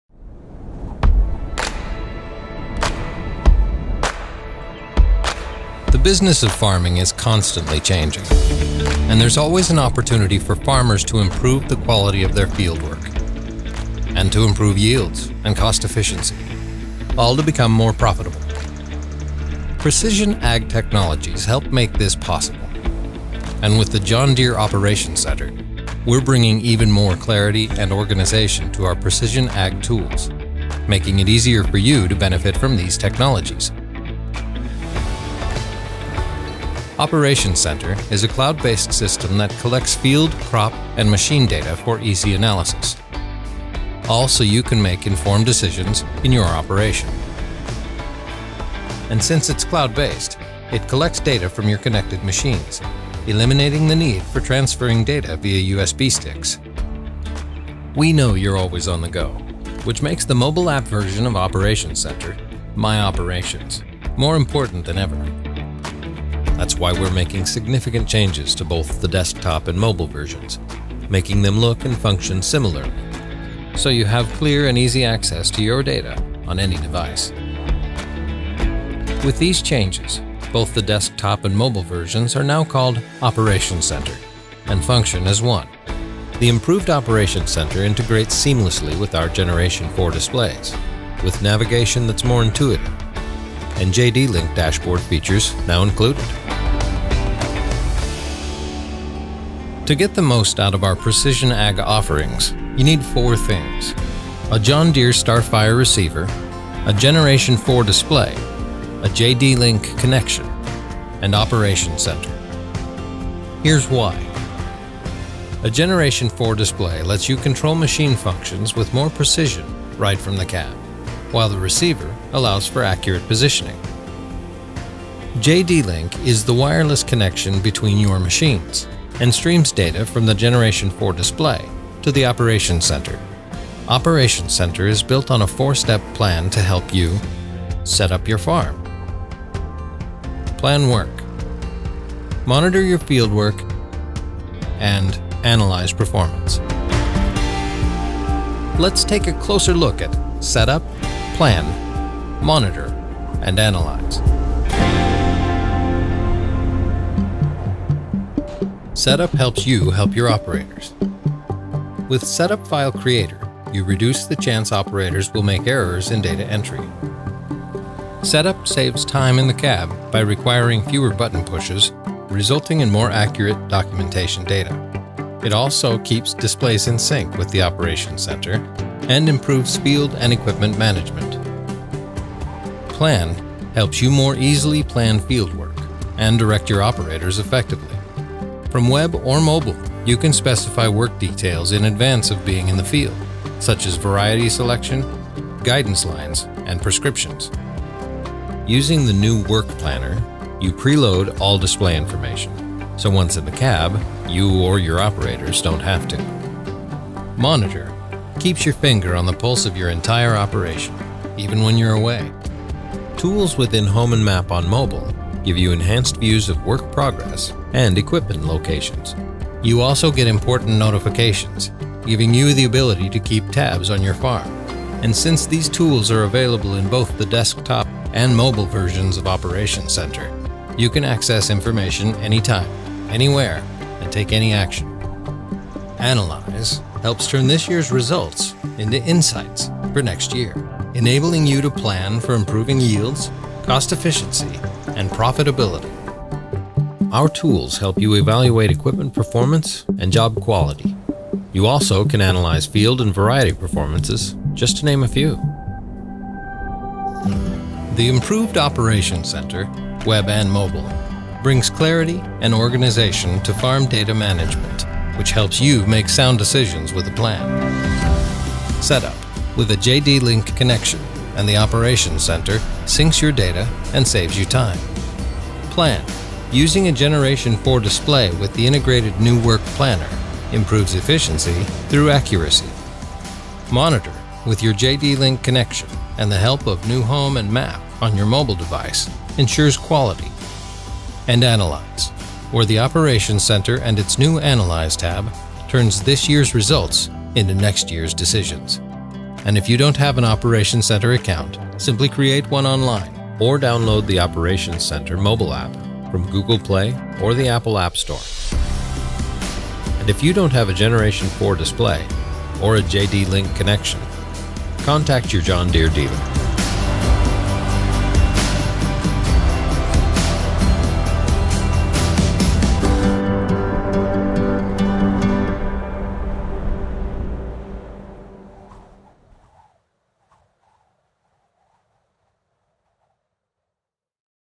January 2021 Updates | John Deere Operations Center
See what’s new in the John Deere Operations Center. For starters, the mobile app version, MyOperations™,  has been renamed Operations Center. Both the web and mobile versions feature a new way to access farm management tools, with tools organized around Setup, Plan, Monitor, and Analyze. The new Work Planner tool can help you streamline setting up field data so your equipment operators don’t have to. And JDLink™ dashboard features are now included in Operations Center.

The improved Operations Center — web and mobile — brings clarity and organization to farm data management. To get started using Operations Center, simply create a free account The iOS version for Apple mobile devices is available for download in the Apple App Store; the Android version will be available in Google Play in the coming weeks.